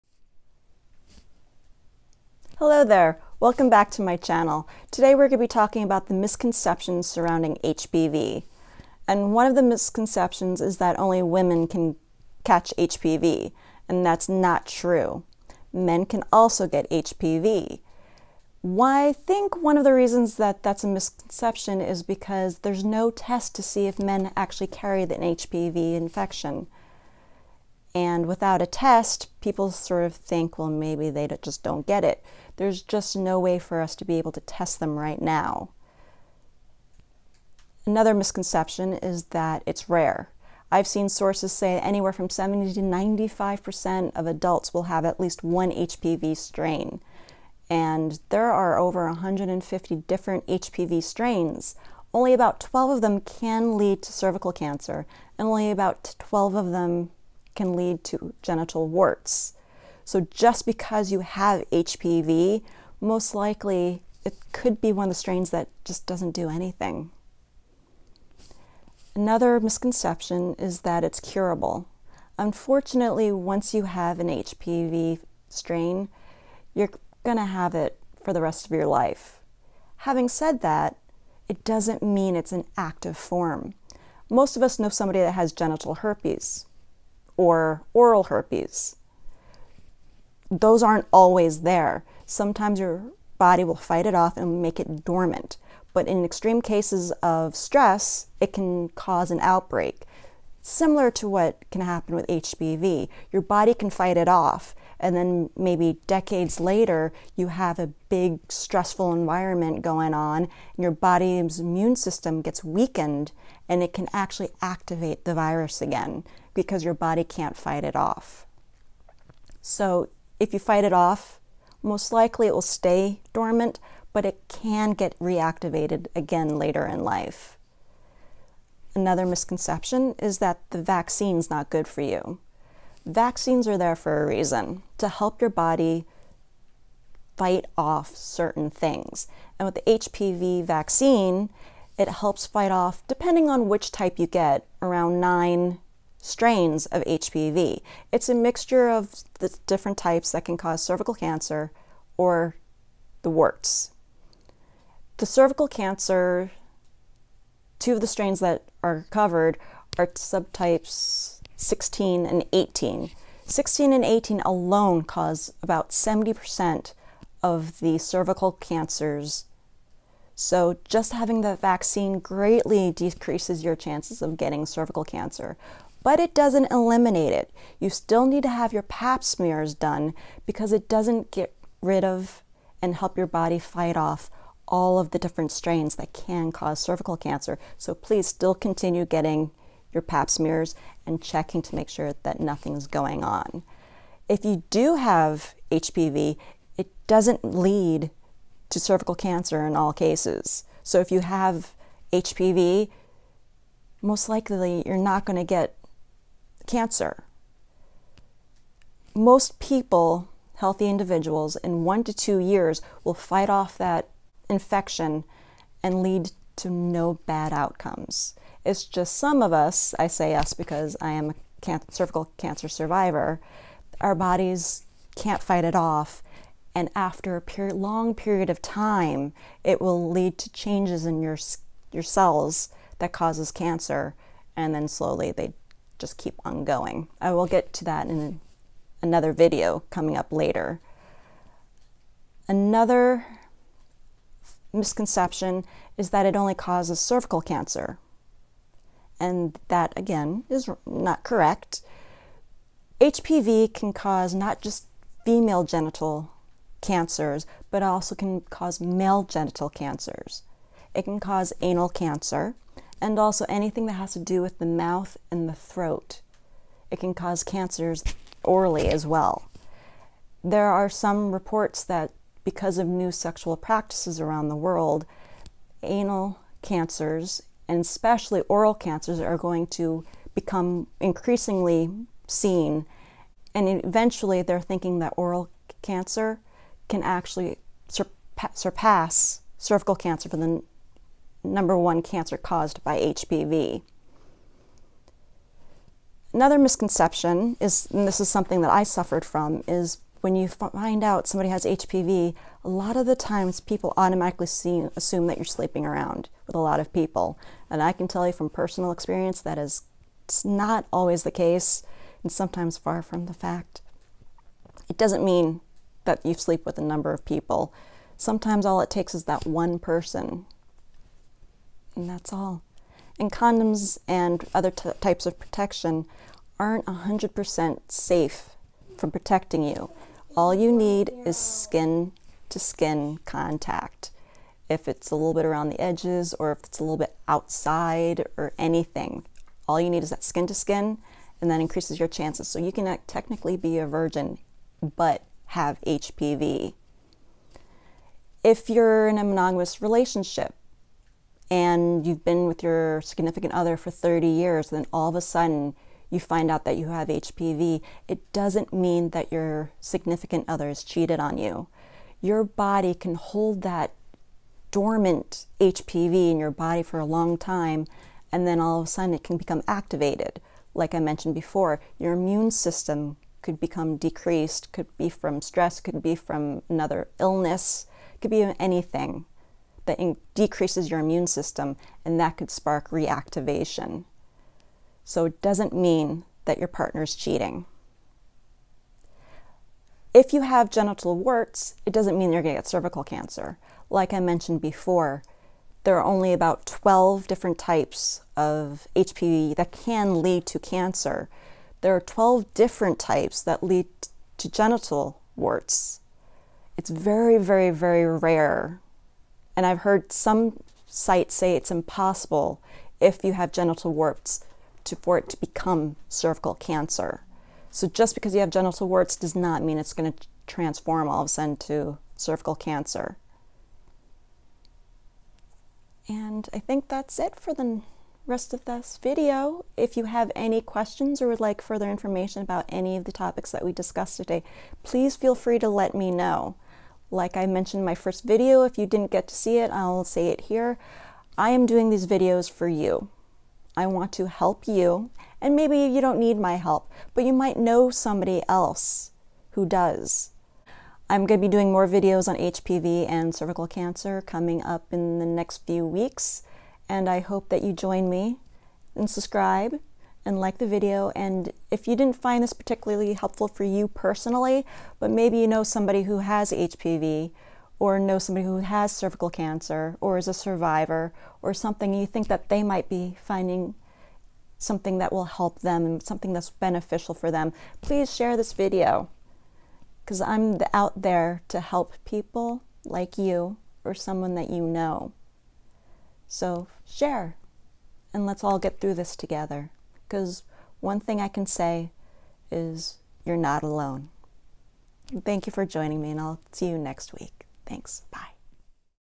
Common misconceptions about HPV.
This video goes over some of the common disbeliefs surrounding HPV. I go over the beliefs that include that only women get it, that men can get tested, that genital warts leads to cancer, that HPV is rare and curable, and more. If you have any concerns of course speak to your doctor.